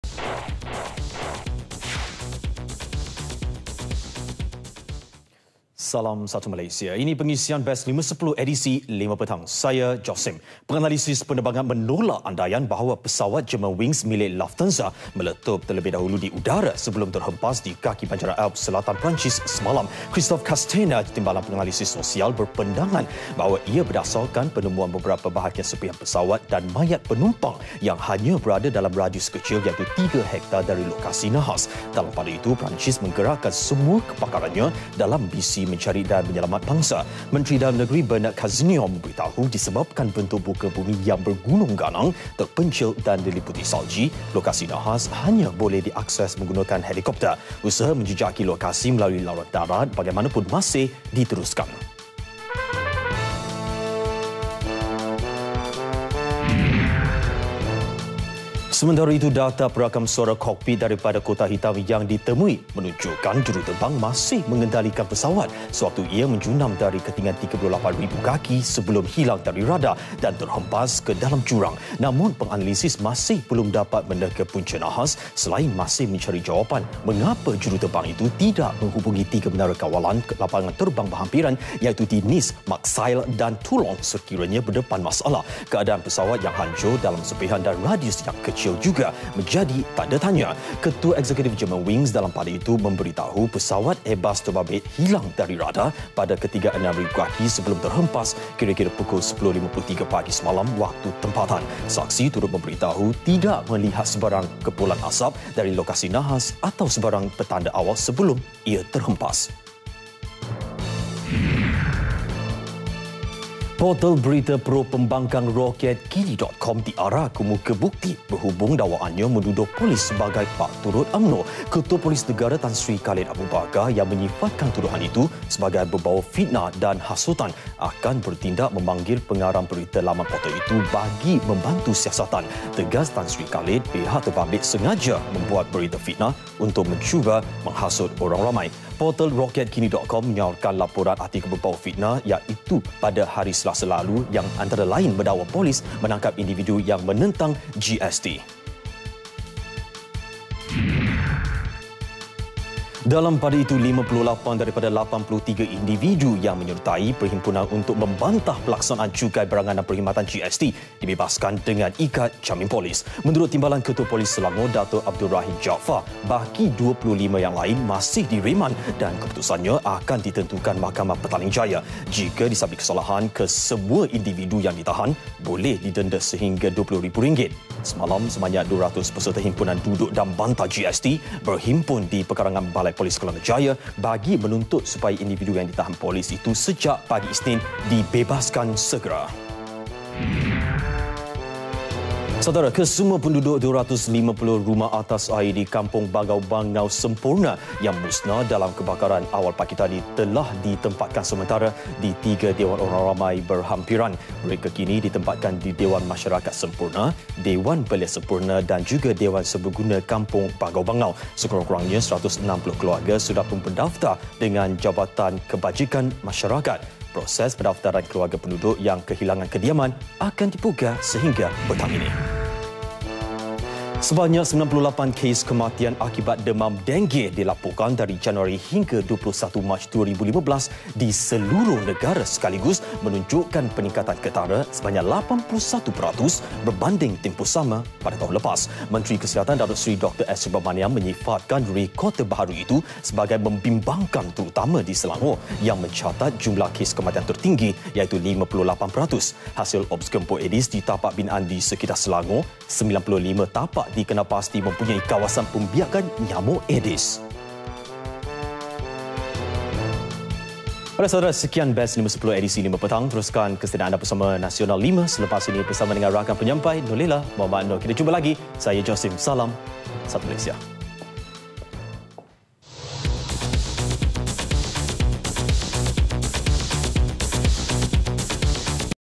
BES 5-10 5 PM [25 MAC 2015]
1. Penganalisis penerbangan MENOLAK andaian, pesawat Germanwings milik Lufthansa, MELETUP TERLEBIH DAHULU DI UDARA.
2. data perakam suara kokpit Germanwings menunjukkan juruterbang masih mengendalikan pesawat.
3. Portal berita pro pembangkang, roketkini.com DIARAH kemuka bukti.
4. 58 daripada 83 individu yang menyertai perhimpunan GST dibebaskan dengan ikat jamin polis.
5. 250 rumah atas air di Kampung Bangau-Bangau, Semporna  yang musnah.
6. 98 kes kematian, akibat demam denggi, dilaporkan dari Januari hingga 21 Mac 2015.